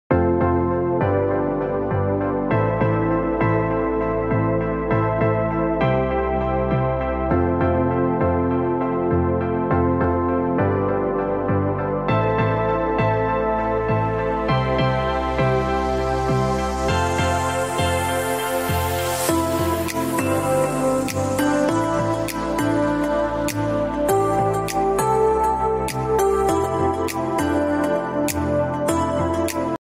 Benefits of ESD Turnstile Access
Our ESD access control system is established to control ESD damages on production floor to improve your product's quality, raise profits and boost your brand image. Our ESD control system is customizable, can be integrated with your existing turnstile gates, flap barrier gates and magnetic gates.